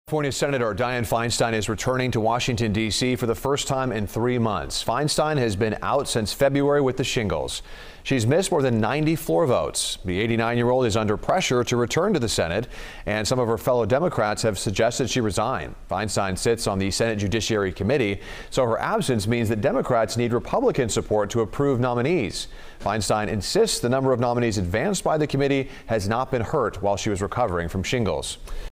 Feinstein returning to Senate after facing resignation calls
Sen. Dianne Feinstein’s office disclosed Tuesday that she is returning to Washington following an absence of more than two months in which the oldest member of Congress faced calls from within her own party to resign.